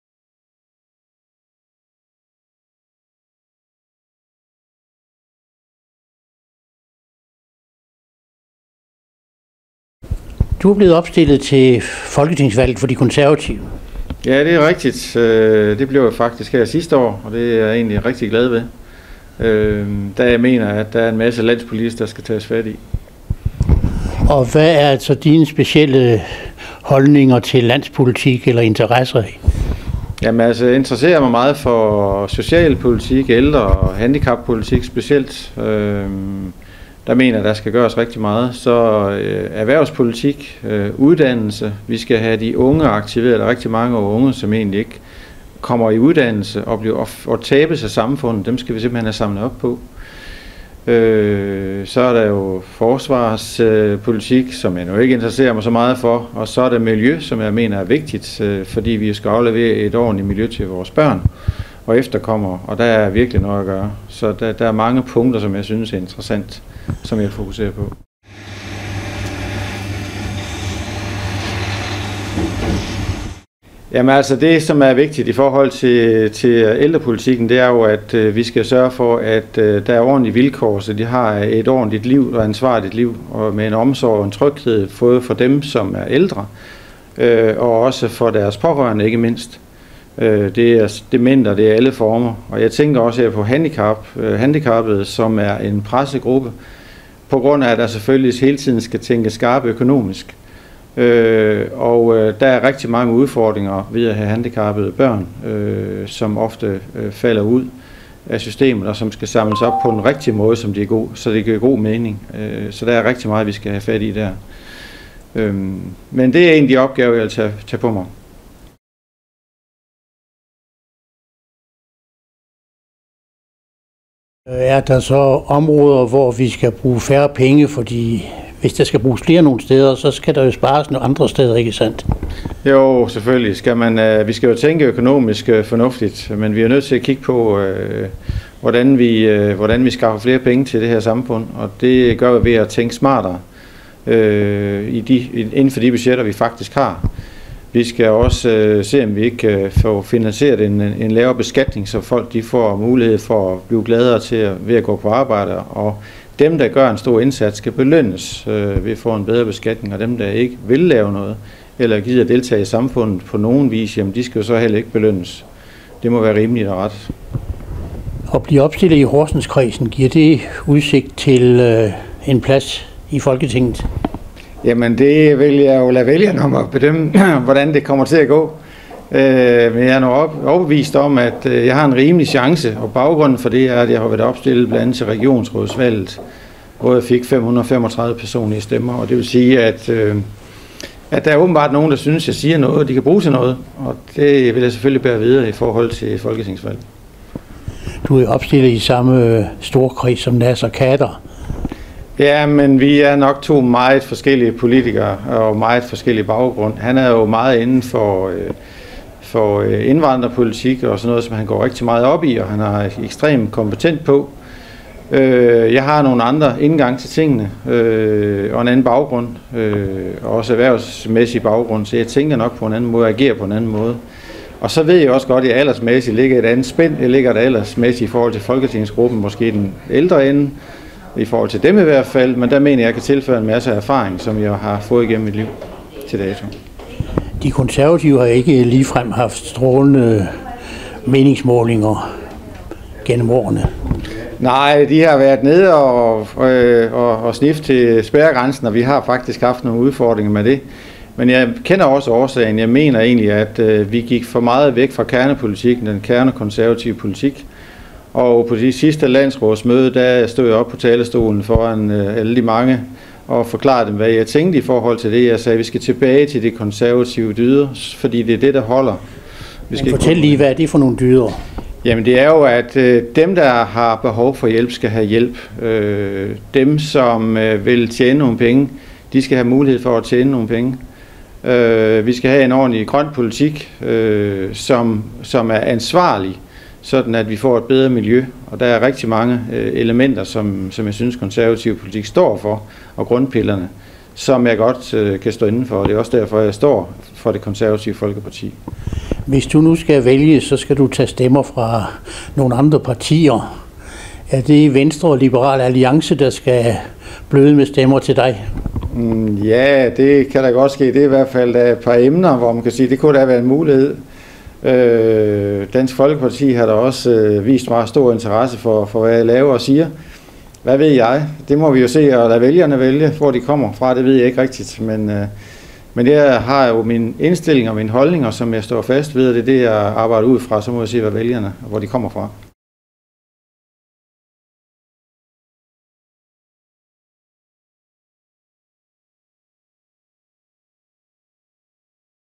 Ny folketingskandidat: Knud Ankjær
Knud Ankjær opstiller i Horsenskredsen for De Konservative